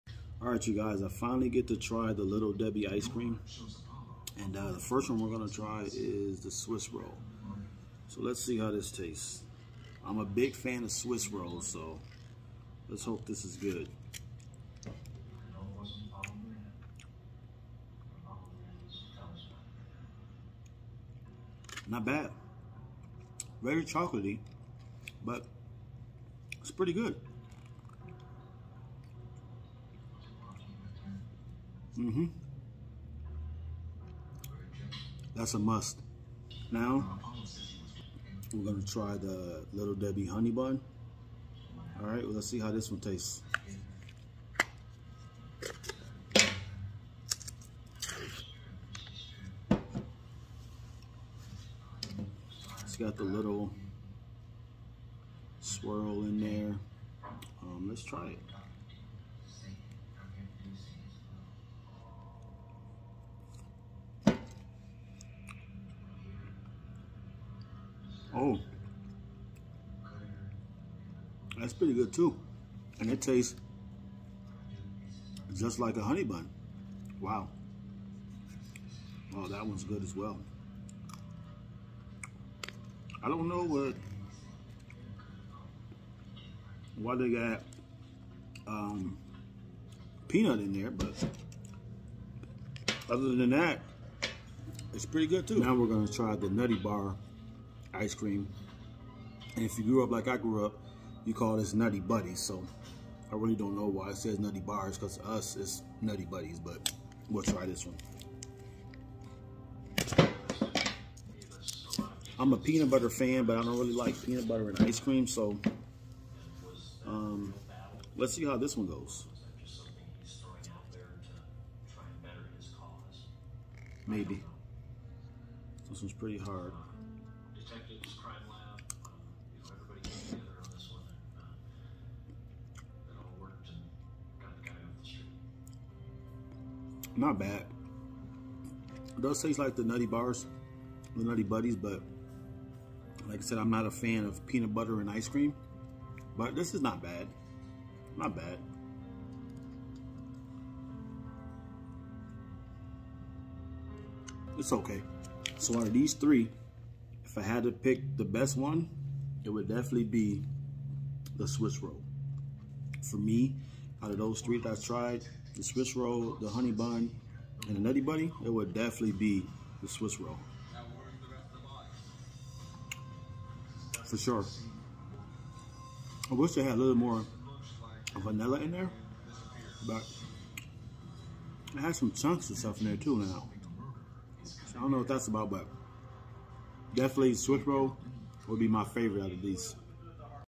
Little Debbie Ice Cream
Finally found the infamous Little Debbie ice cream so I'm trying 3 of them out for the first time!!!